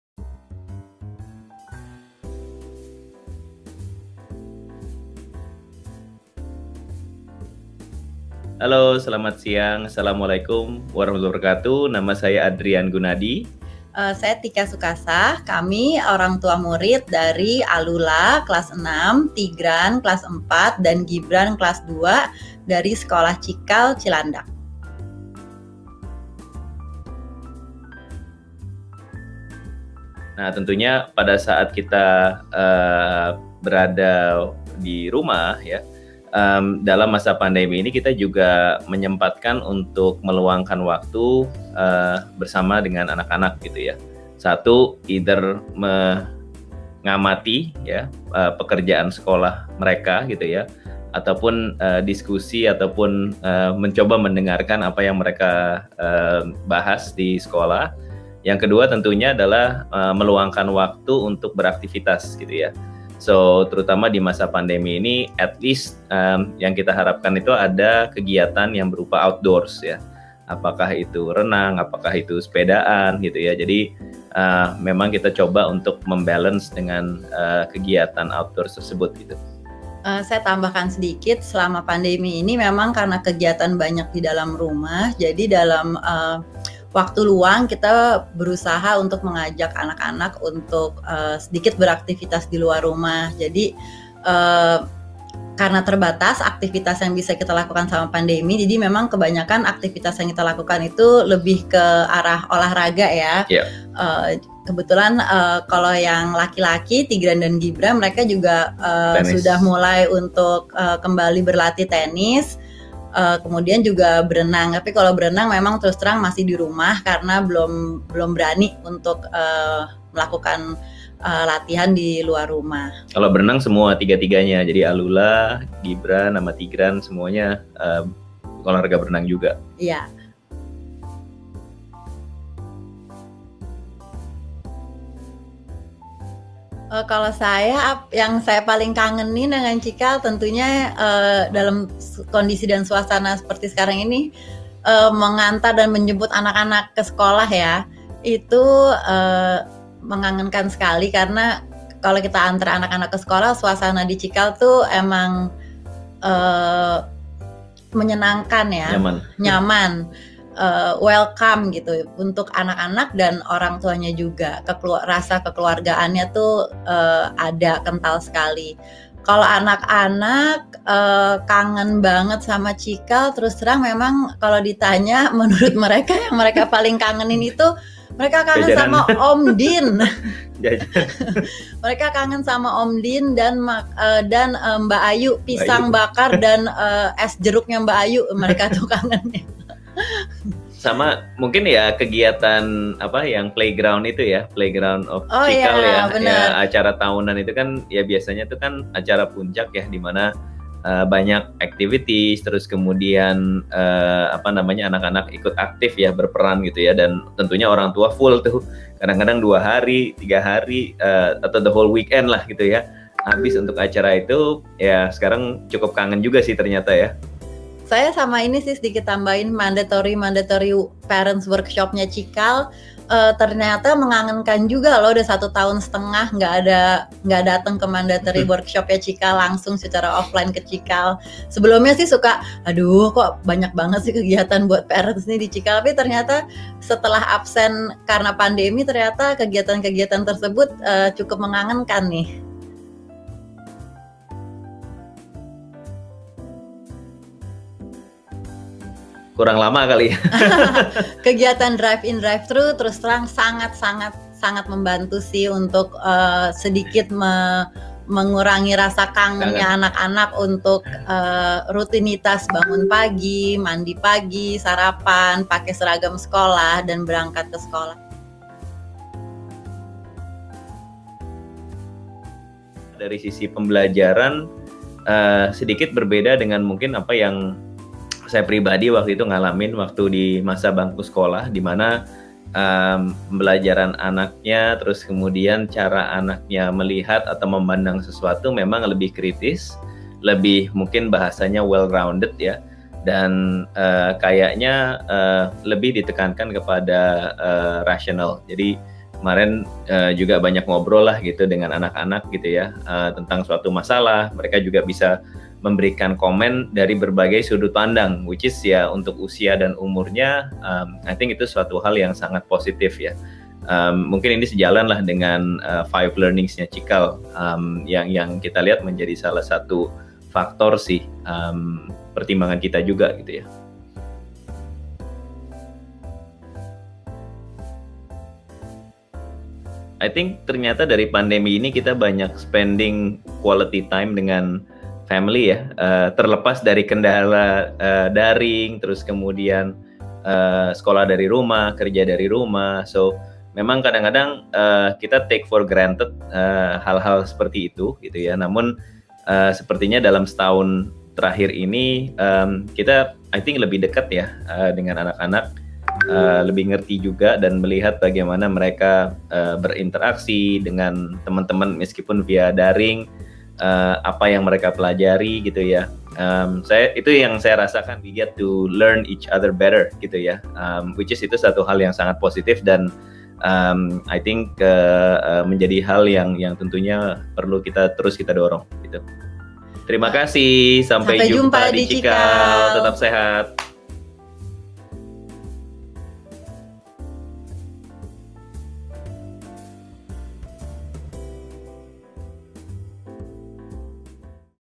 Cerita Keluarga Cikal: "Pembelajaran di Cikal Dukung Anak Kami Jadi Well Rounded and Rational!"
Pandemi membuat banyak keluarga Cikal merindukan sekolah dan interaksi yang dibangun di komunitas Cikal. Cikal pun rindu sekali! Kira-kira apa yang paling Papa, dan Mama rindukan dari Sekolah Cikal?

Di episode CeritaKeluargaCikal terbaru ini, Sekolah Cikal senang sekali dapat mengundang dan berbincang seru dengan salah satu keluarga Cikal, yakni Bapak Adrian Gunadi, dan Ibu Tika Soekasah orang tua murid dari Alulla, Tigran, dan Gibran, ketiganya adalah murid Sekolah Cikal Cilandak. Di bincang-bincang ini, Bapak Adrian dan Ibu Tika juga bercerita hal yang paling dirindukan dari Sekolah loh! Apa ya kira-kira?

**Sesi bincang-bincang Keluarga Cikal ini dilakukan sebelum Moving up ceremony. Kini, Alulla, Tigran dan Gibran akan melanjutkan perjalanan pendidikan bermakna dan menyenangkan di tahun ajaran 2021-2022 bersama Cikal!